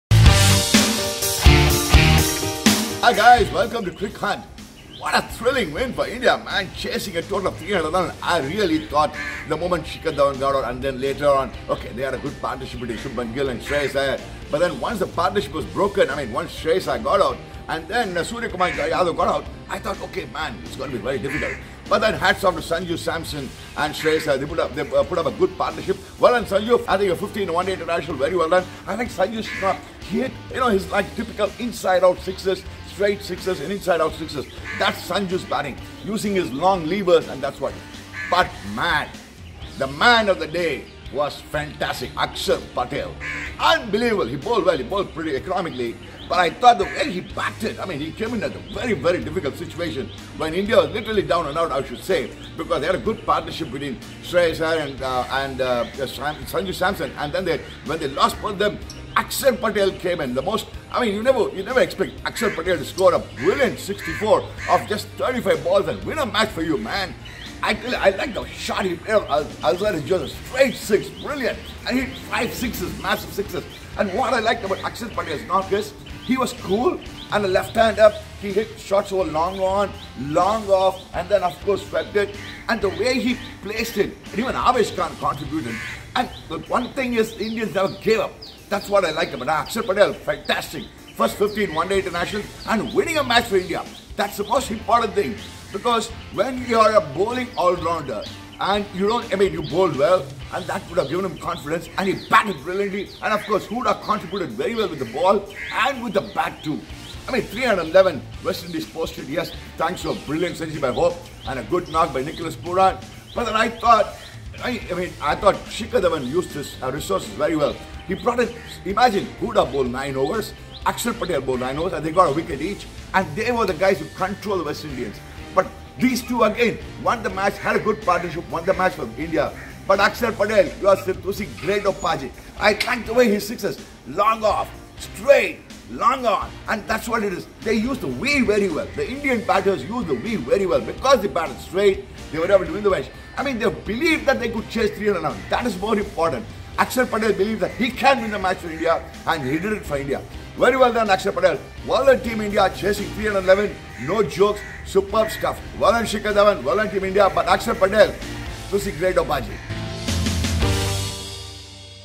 IND vs WI: 2nd ODI मैच पर Krishnamachari Srikkanth की राय | वनइंडिया हिंदी *Cricket
IND vs WI पर Exclusive कृष्णमाचारी श्रीकांत विश्व कप विजेता टीम के सदस्य की राय..भारत और वेस्ट इंडीज के बीच दूसरा ODI मैच 24 जुलाई रविवार को Queens Park Oval क्रिकेट स्टेडियम में खेला गया था